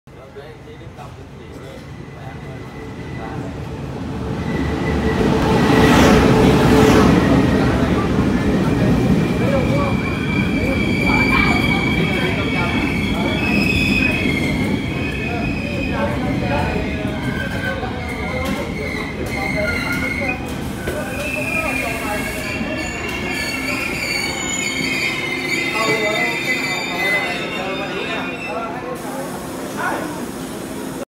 128次自強號往基隆進入新竹站
拍攝時間：2021/3/3 17:18
車號：(屏東.高雄.彰化)E1061+E1039(台北.基隆)